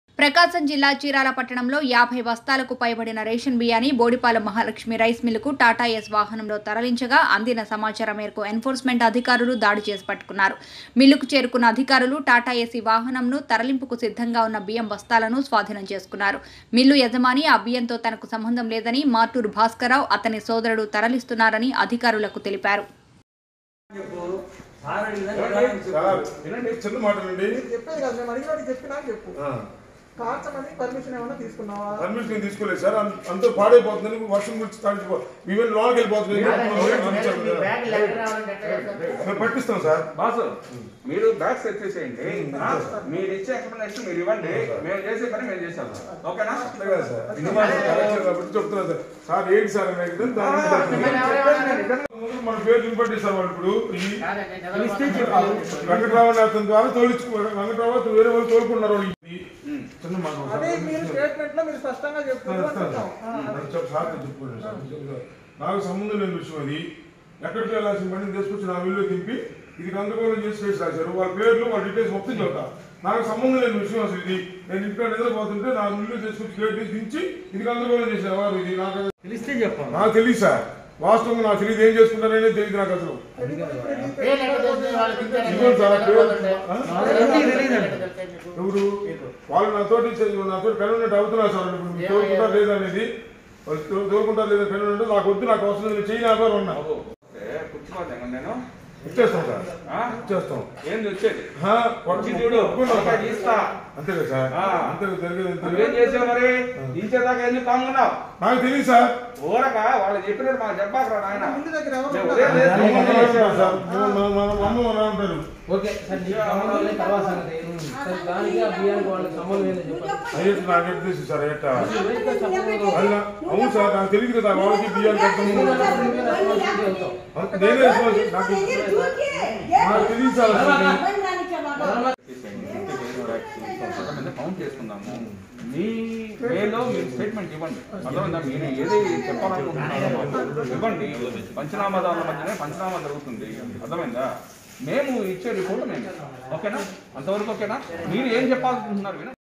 చీరాలలో అక్రమంగా తరలిస్తు్న్న రేషన్‌ బియ్యం - పట్టుకున్న ఎన్‌ఫోర్స్‌మెంట్‌ అధికారులు || DN5 News
ప్రకాశం జిల్లా చీరాల పట్టణంలో యాభై బస్తాలకు పైబడిన రేషన్ బియ్యాన్ని బోడిపాలెం మహాలక్ష్మి రైస్ మిల్లుకు టాటా ఏస్ వాహనంలో తరలించగా అందిన సమాచారం మేరకు ఎన్ఫోర్స్ మెంట్ అధికారులు దాడి చేసి పట్టుకున్నారు. మిల్లుకు చేరుకున్న అధికారులు టాటా ఏసి వాహనంను, తరలింపుకు సిద్ధంగా ఉన్న బియ్యం బస్తాలను స్వాధీనం చేసుకున్నారు. మిల్లు యజమాని ఆ బియ్యంతో తనకు సంబంధం లేదని, మార్టూరు భాస్కరరావు, అతని సోదరుడు తరలిస్తున్నారని అధికారులకు తెలిపారు.